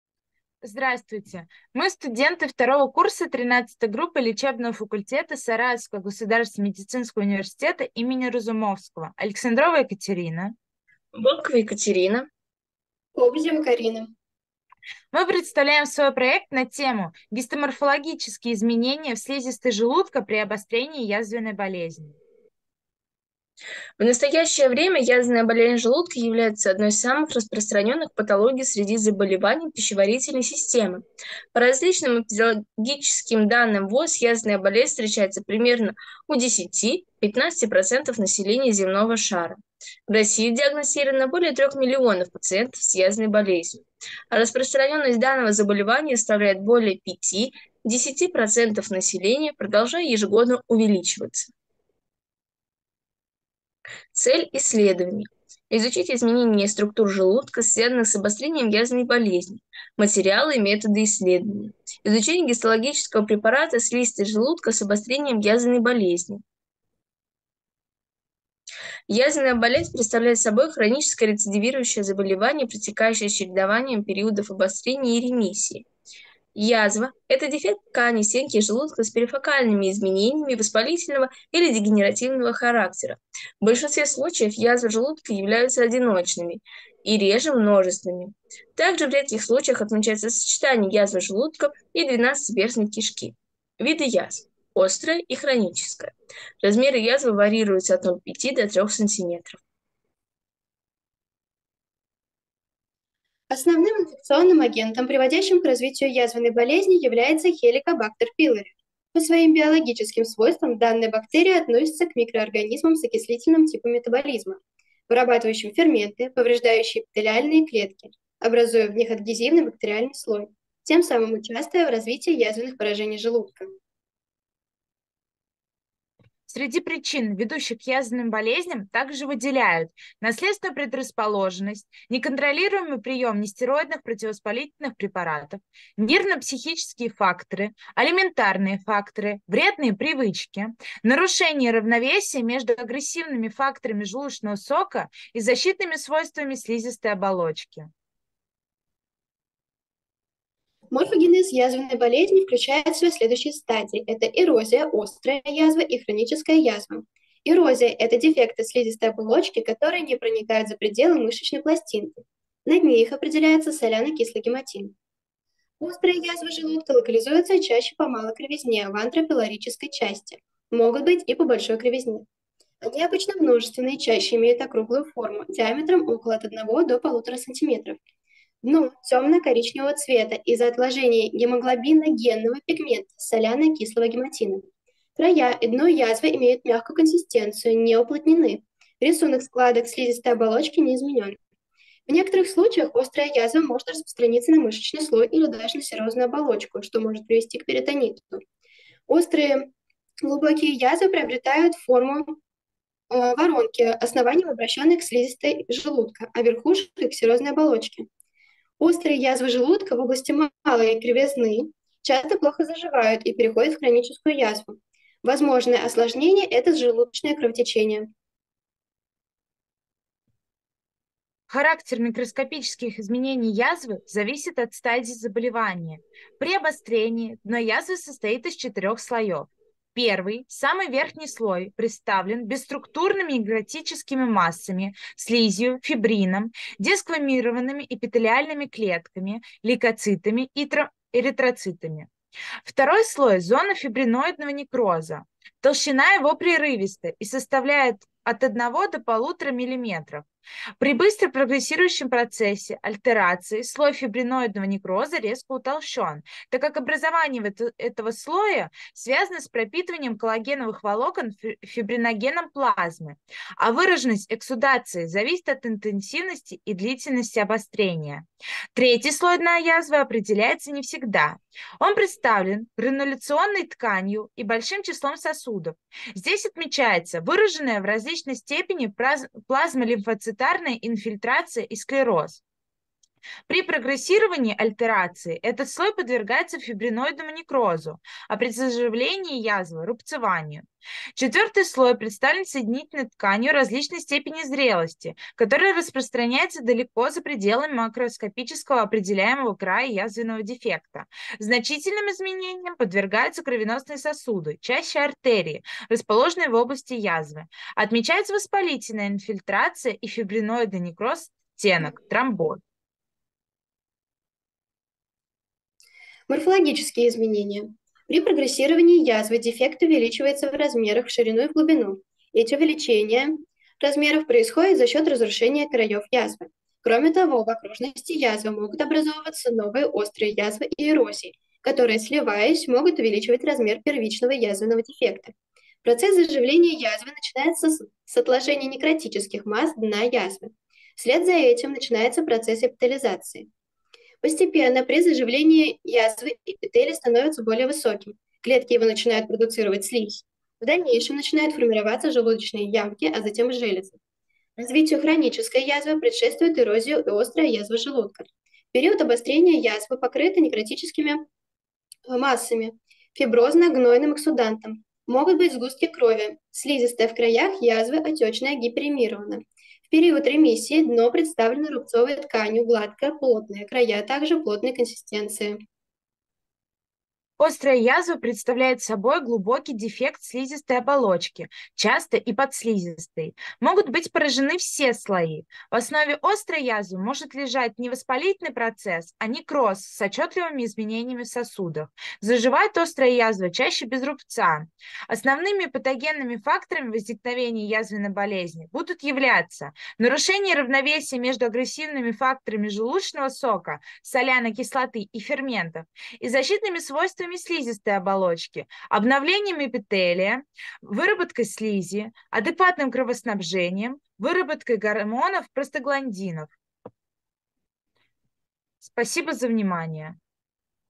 7. ГИСТОМОРФОЛОГИЧЕСКИЕ ИЗМЕНЕНИЯ В СЛИЗИСТОЙ ЖЕЛУДКА ПРИ ОБОСТРЕНИИ ЯЗВЕННОЙ БОЛЕЗНИ
7. ГИСТОМОРФОЛОГИЧЕСКИЕ ИЗМЕНЕНИЯ В СЛИЗИСТОЙ ЖЕЛУДКА ПРИ ОБОСТРЕНИИ ЯЗВЕННОЙ БОЛЕЗНИ
Александрова Екатерина Васильевна – студ. 2 курса леч. фак. СГМУ им. В.И. Разумовского.
Волкова Екатерина Михайловна – студ. 2 курса леч. фак. СГМУ им. В.И. Разумовского.
Кобызева Карина Сергеевна – студ. 2 курса леч. фак. СГМУ им. В.И. Разумовского.
Научный руководитель: Уварова Ирина Александровна, канд. биол. наук, доцент кафедры гистологии СГМУ В.И. Разумовского.